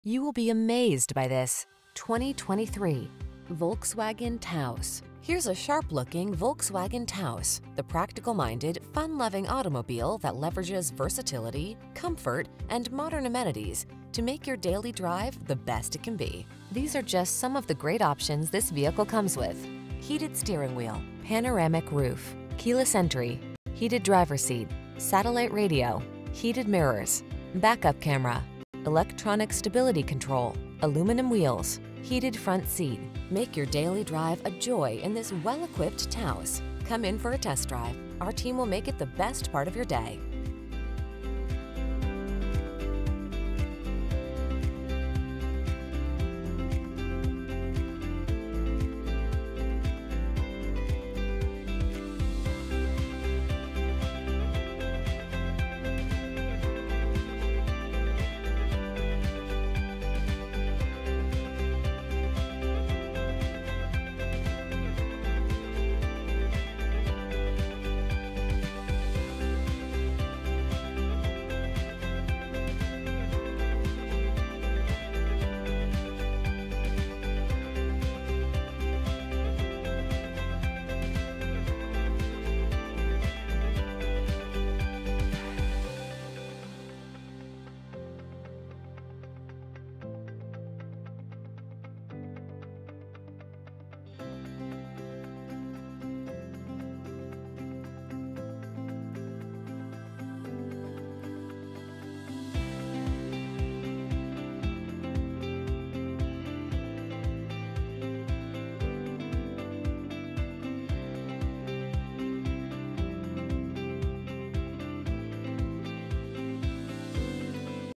2023 Volkswagen Taos Waco, Hewitt, Woodway, Robinson, Belmead PM356256
Pure Gray w/Black Roof New 2023 Volkswagen Taos 1.5T SE available in 2301 W. Loop 340, Waco, Texas at VW of Waco. Servicing the Waco, Hewitt, Woodway, Robinson, Belmead, TX area.

VW of Waco
2301 W. Loop 340

Pure Gray w/Black Roof 2023 Volkswagen Taos 1.5T SE FWD 8-Speed Automatic with Tiptronic 1.5L I4 Turbocharged DOHC 16V LEV3-SULEV30 158hpbrbr28/36 City/Highway MPG